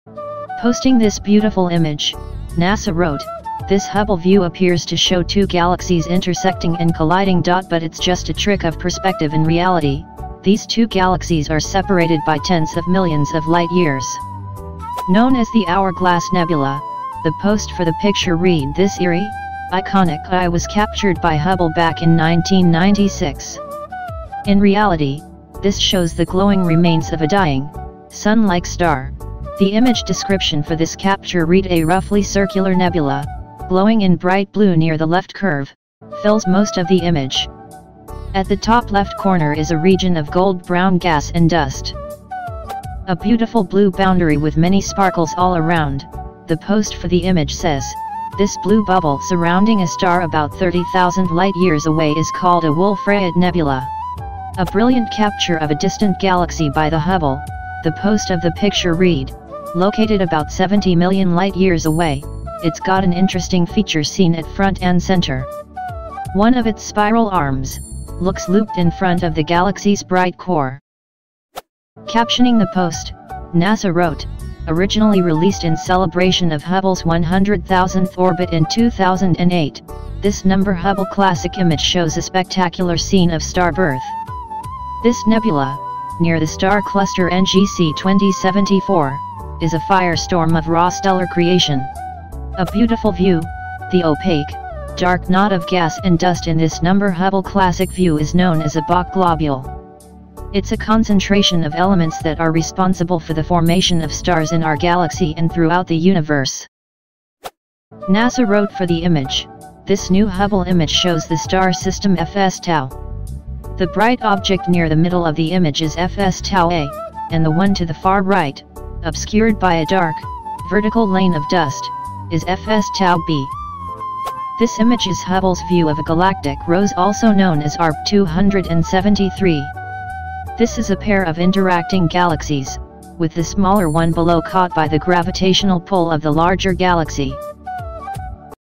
Spectacular pictures captured by NASA’s Hubble space telescope
Posting this beautiful image, NASA wrote, "This Hubble view appears to show two galaxies intersecting and colliding.But it’s just a trick of perspective – in reality, these two galaxies are separated by tens of millions of light-years."
 -Known as the Hourglass Nebula, the post for the picture read "This eerie, iconic “eye” was captured by Hubble back in 1996. In reality, this shows the glowing remains of a dying, Sun-like star."
 -The image description for this capture read "A roughly circular nebula, glowing in bright blue near the left curve, fills most of the image. At the top left corner is a region of gold-brown gas and dust."
 -A beautiful blue boundary with many sparkles all around, the post for the image says, "This blue "bubble" surrounding a star about 30,000 light-years away is called a Wolf-Rayet nebula."
 -A brilliant capture of a distant galaxy by the Hubble, the post of the picture read, "Located about 70 million light-years away, it's got an interesting feature seen at front-and-center. One of its spiral arms, looks looped in front of the galaxy's bright core."
-Captioning the post, NASA wrote, "Originally released in celebration of Hubble's 100,000th orbit in 2008, this HubbleClassic image shows a spectacular scene of starbirth. This nebula, near the star cluster NGC 2074, is a firestorm of raw stellar creation."
-A beautiful view, "The opaque, dark knot of gas and dust in this HubbleClassic view is known as a Bok globule. It's a concentration of elements that are responsible for the formation of stars in our galaxy and throughout the universe!"
 -NASA wrote for the image, "This new Hubble image shows the star system FS Tau. The bright object near the middle of the image is FS Tau A, and the one to the far right (obscured by a dark, vertical lane of dust) is FS Tau B."
 -"This image is Hubble's view of a galactic "rose" – also known as Arp 273. This is a pair of interacting galaxies, with the smaller one below caught by the gravitational pull of the larger galaxy."